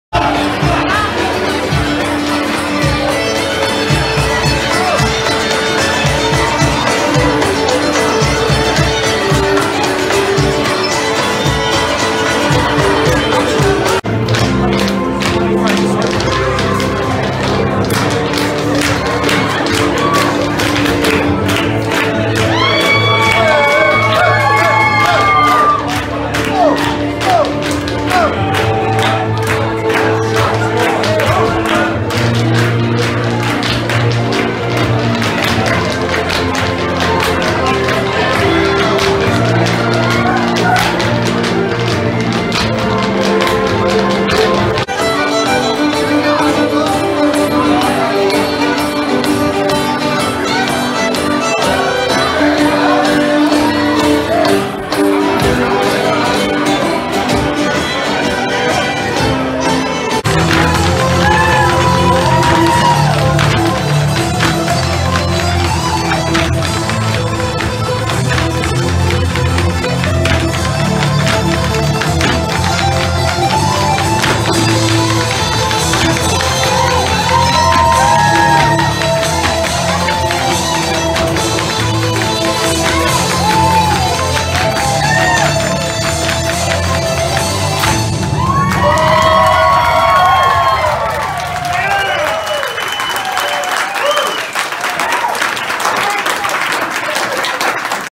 St. Patrick's Day in Lancaster
With the city’s pubs crowded and money flowing freely, students from local Irish dance schools often stop by to perform Irish step dancing and pass the hat while people are celebrating St. Patrick’s Day in Lancaster, Pennsylvania, USA.  These clips were filmed at Tellus360 on March 14, 2020 and Meduseld Meadery on March 17, 2019.